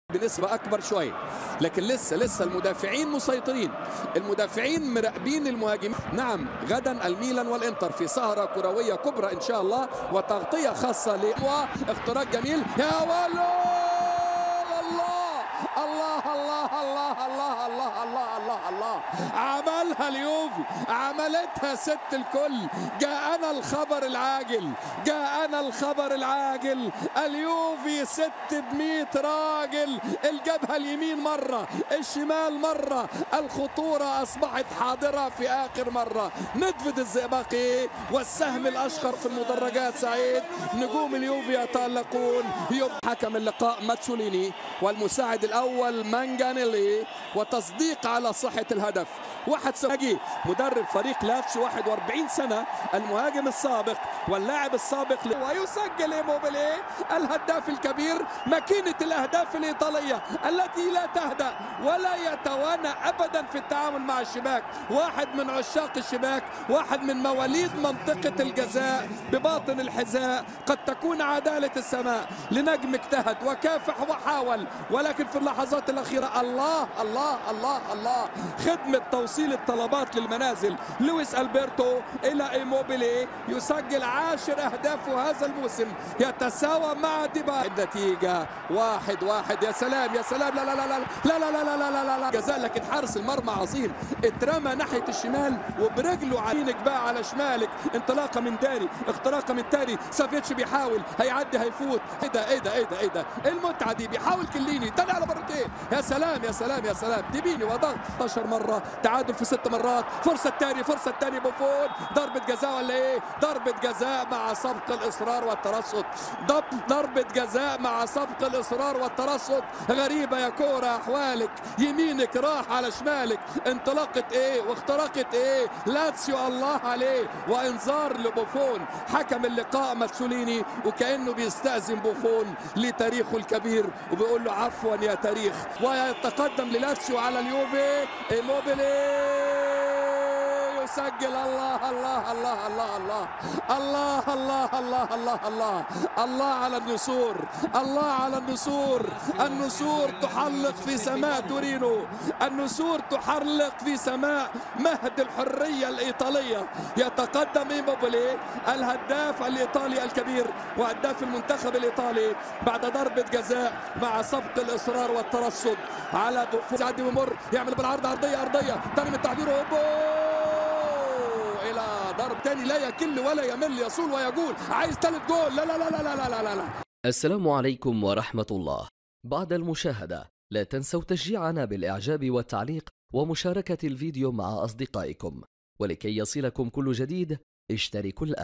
ملخص اهداف مباراة يوفنتوس ولاتسيو 1 2 شاشة كاملة الدوري الإيطالي
ملخص اهداف مباراة يوفنتوس ولاتسيو